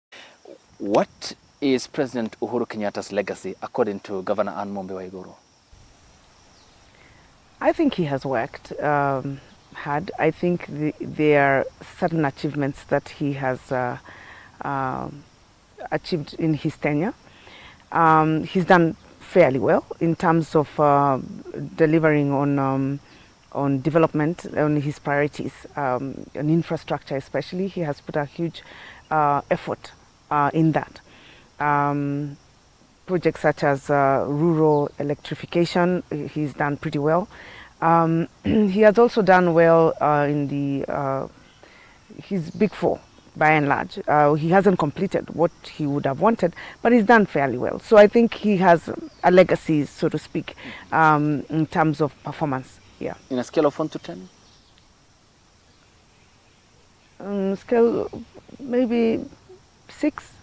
H.E Anne Waiguru - "President Uhuru has done fairly well on development and infrastructure."
H.E Anne Waiguru: I think President Uhuru has worked and has done fairly well in terms of delivering on development and infrastructure. He has also done well in his big four agenda although he hasn't completed it.